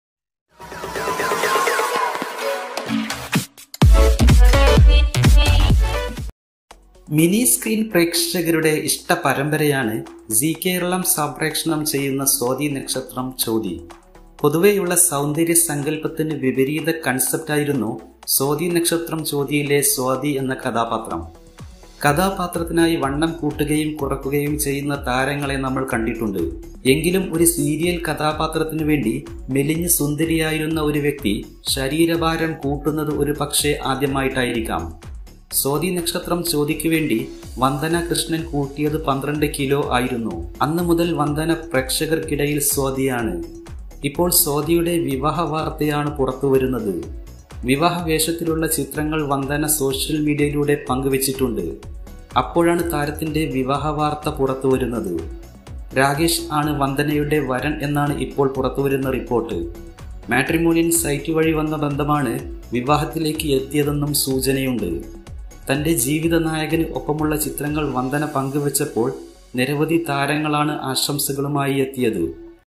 Vandhana Krishnan Marriage | Swathi Nakshthram Chothi Swathi wedding | Husband
സ്വാതി നക്ഷത്രം ചോതിയിലെ സ്വാതി വിവാഹിതയായി വന്ദനയുടെ വിവാഹ ചിത്രങ്ങൾ വൈറൽ
 Malayalam actress Vandhana Krishnan Marriage Swathi Nakshthram Chothi Swathi wedding photos Husband  Vandhana Wedding latest news episode